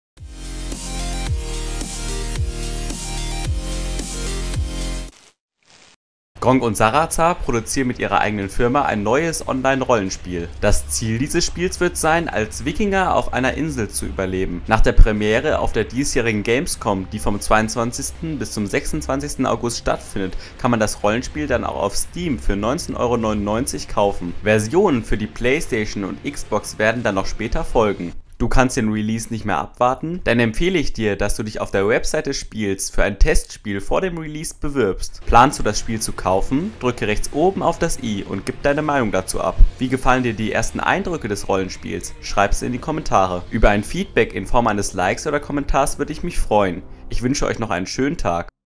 GRONKH & SARAZAR produzieren eigenes Spiel | Valnir Rok | YOUTUBE NEWS
Thema:
- Gronkh und Sarazar produzieren eigenes Spiel